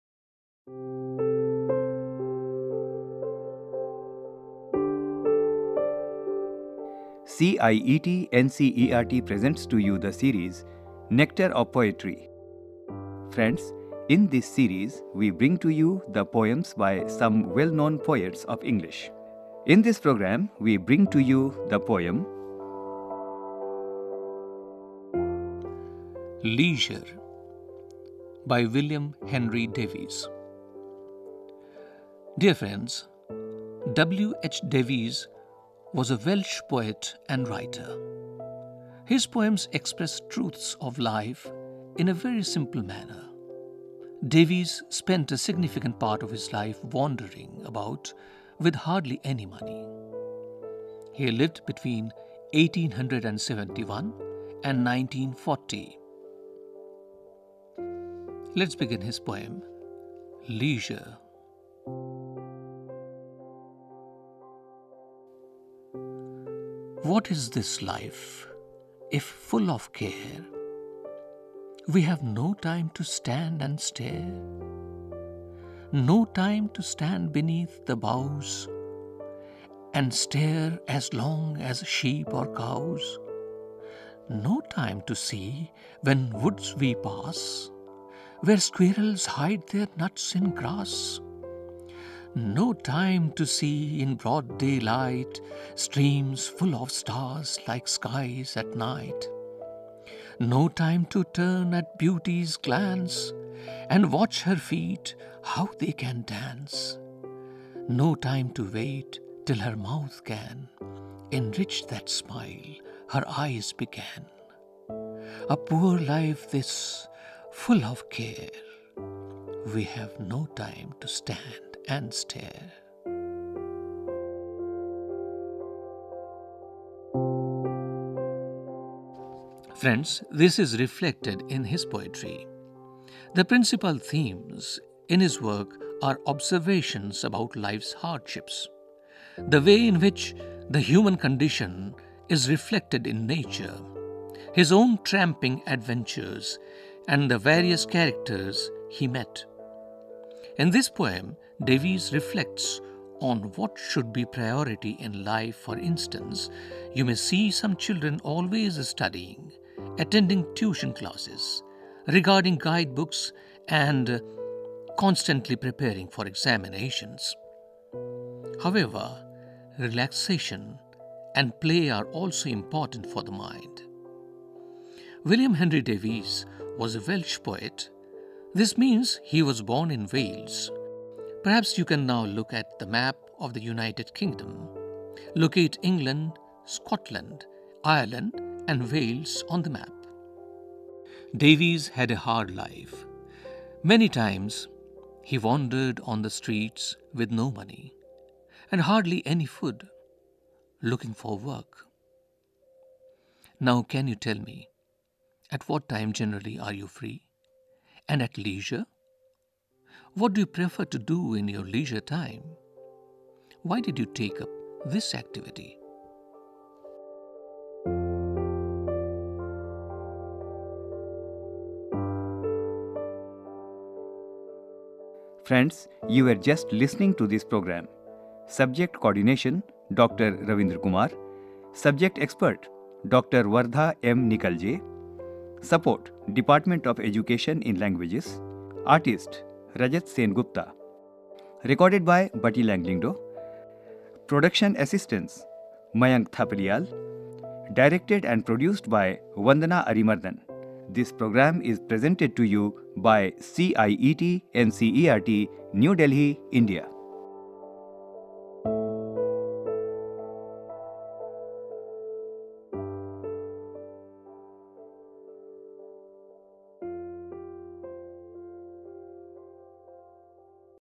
Leisure
This program is the recitation of poem written by William Henry Davis in which he talks about the demerits of a busy life where we have no time to see the beauty of nature.